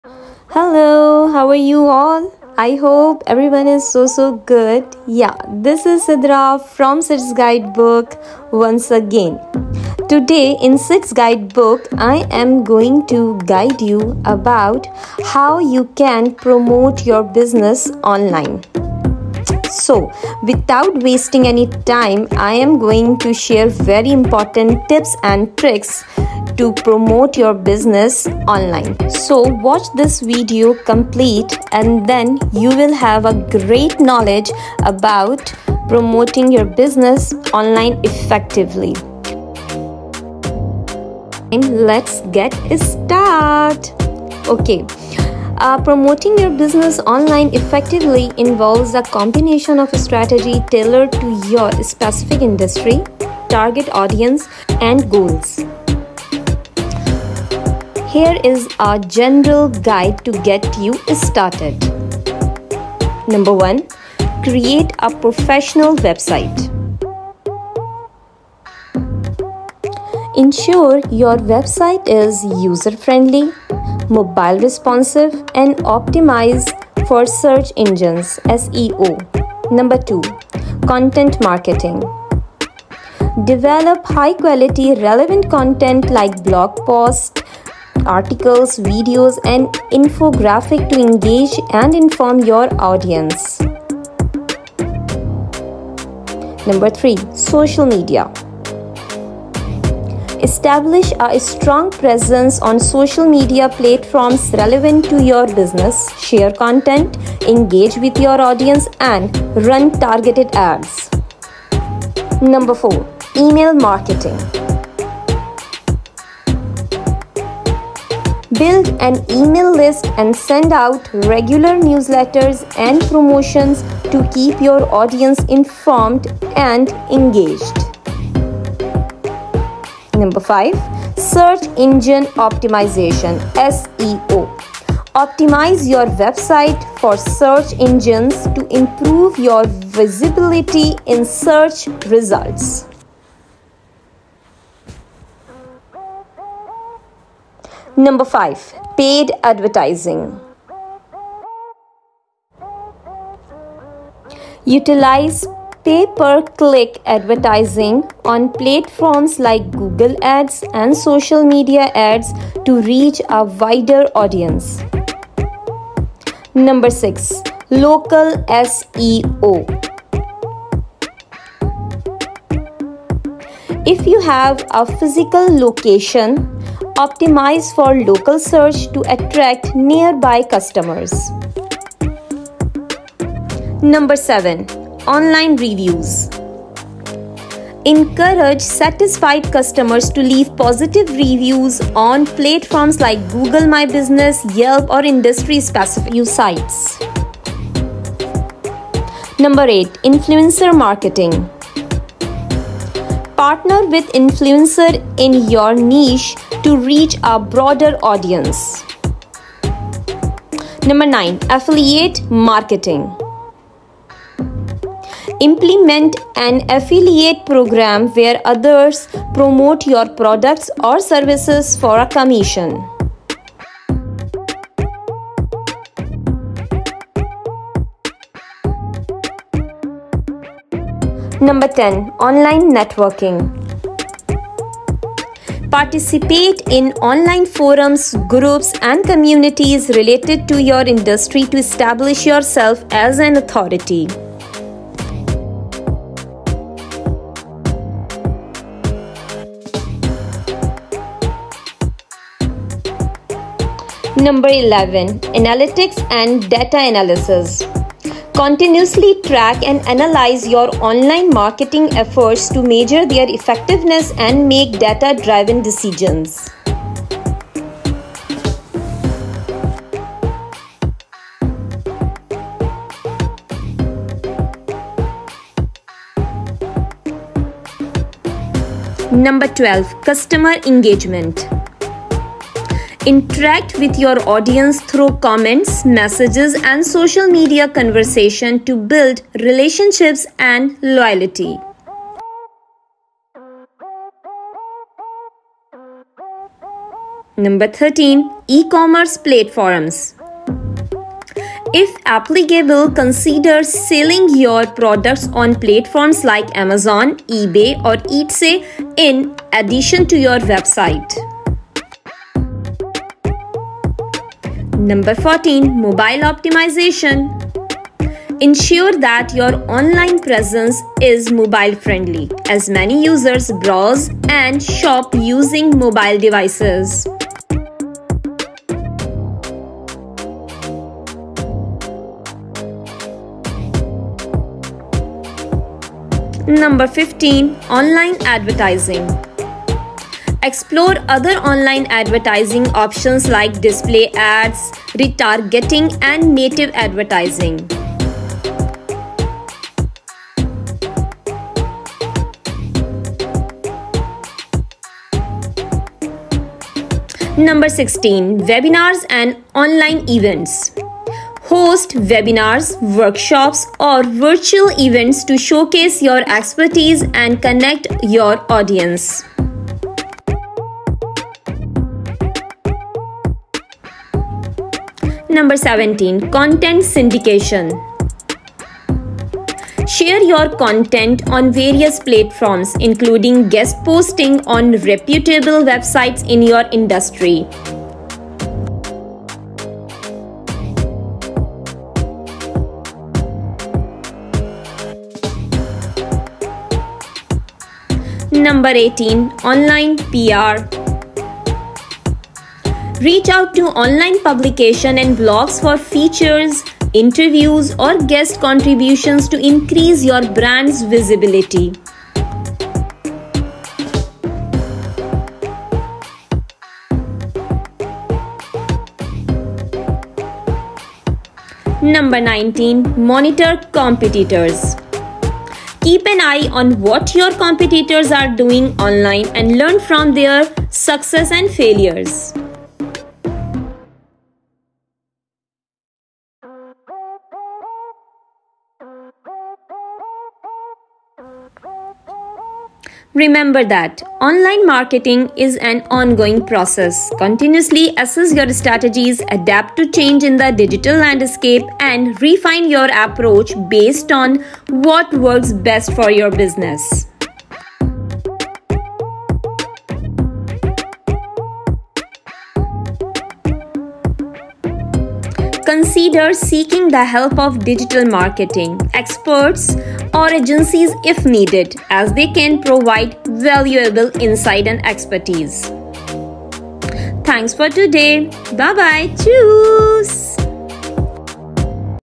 Promote your Business Online | Complete Guidance for online business promotion
hello viewers today I am sharing with you an effective guidance for online business promotion
you will find it helpful I am sure